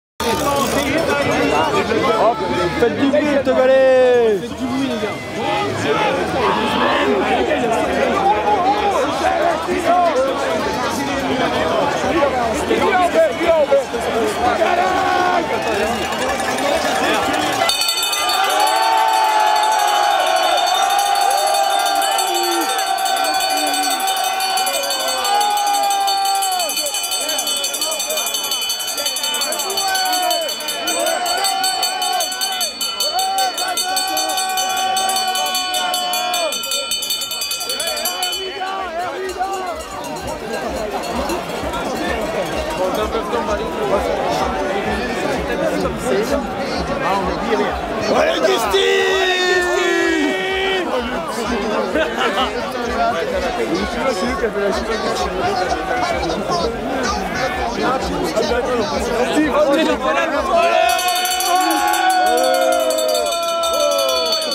Championnats du Monde Cyclo cross St-Wendel 2011 1er Tour
Grosse ambiance dans le 1er tour des championnats du Monde !